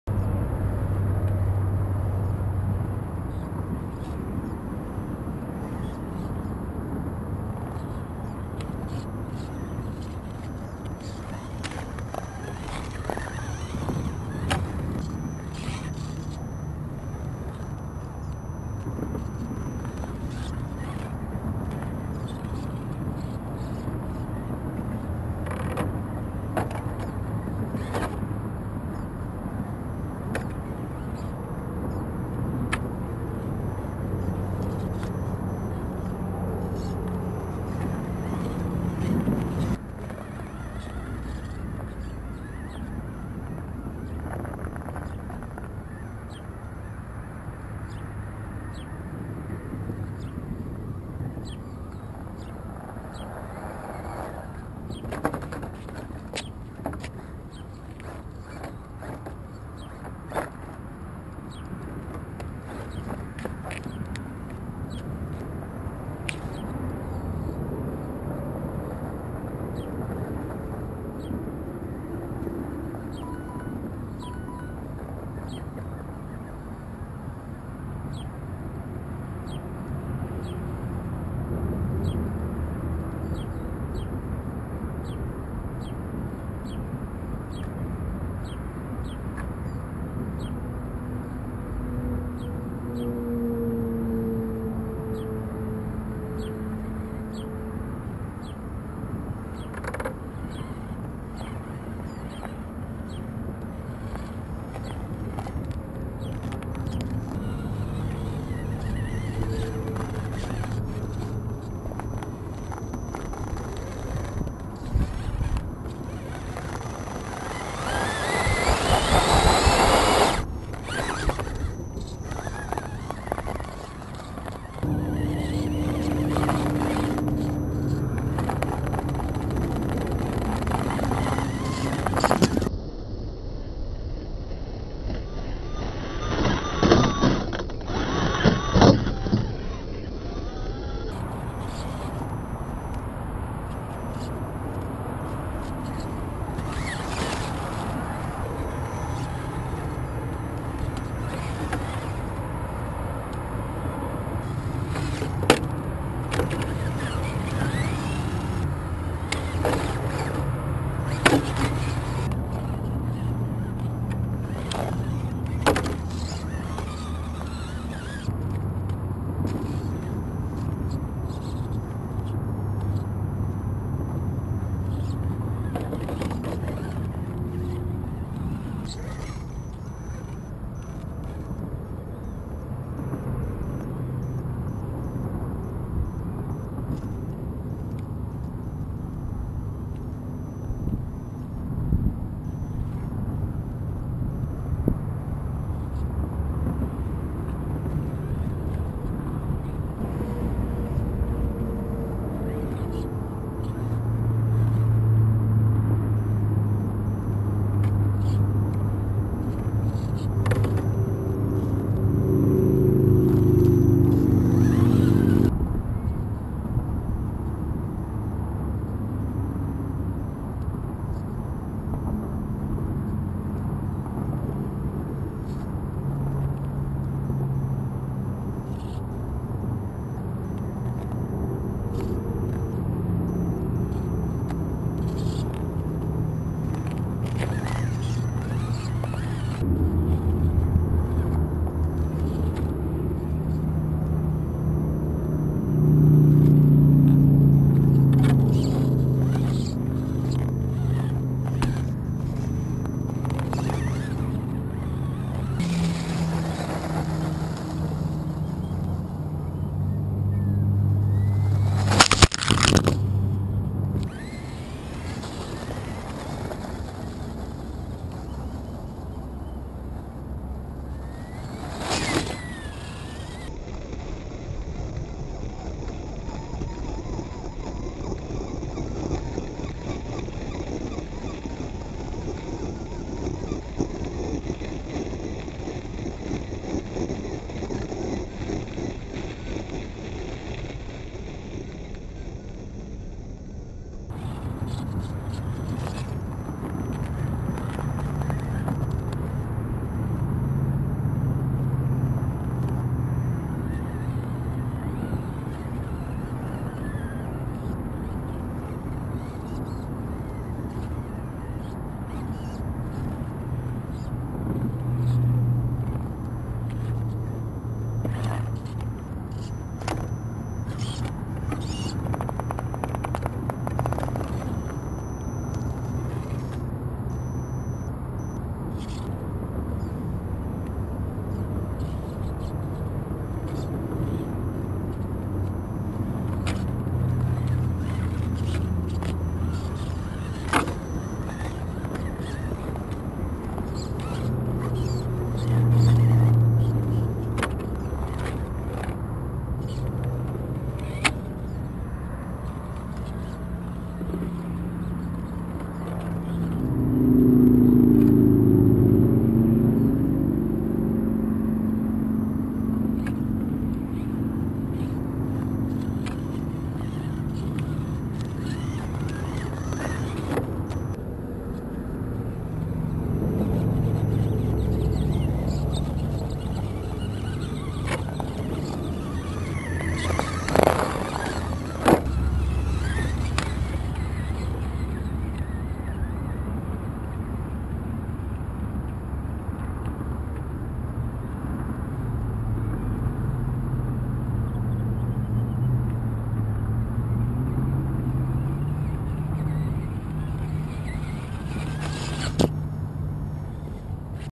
Trail Honcho Axial
First time driving the Axial Trail Honcho. It was pretty fun right out
of the box. Local park here in AZ